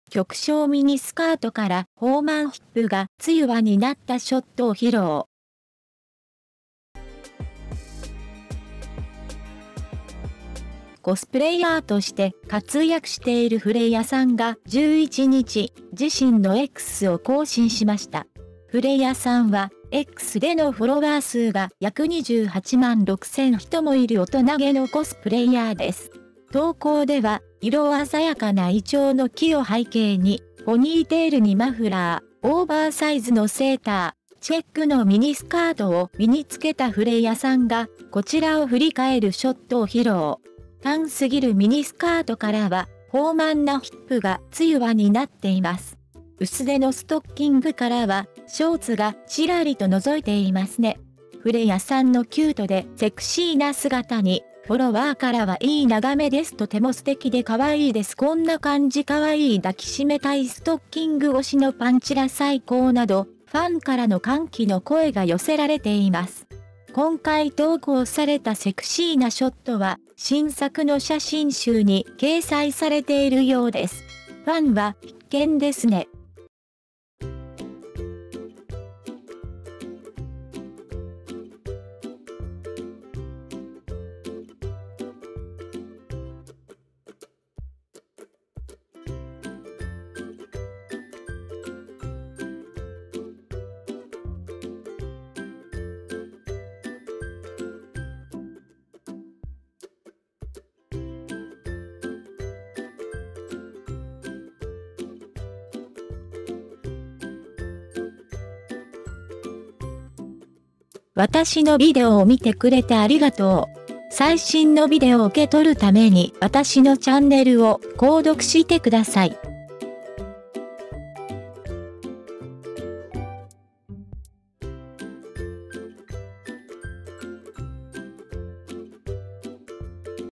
ふれいあ、極小ミニスカートから豊満ヒップが露わになったショットを披露
The post shows a shot of Fureia wearing a ponytail, scarf, oversized sweater, and checked miniskirt against a background of colorful ginkgo trees, looking back at us.
Her too-short miniskirt reveals her ample hips. Her panties are peeking out from her thin stockings.
Fureia's cute and sexy appearance has been met with delightful comments from fans, such as "Nice view," "Very nice and cute," "Cute like this," "I want to hug her," and "Great panties through the stockings.
*フリー素材
背景動画：Pixaday(フリー動画）